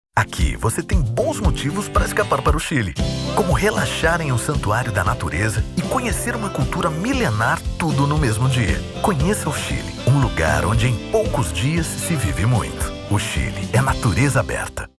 Turismo comercial do Chile: Motivos para escapar ao Chile - Sur
Aquí você tem bons motivos para escapar para o Chile. Conhesca o sul do Chile, un lugar ounde em poucos días, se vive muito.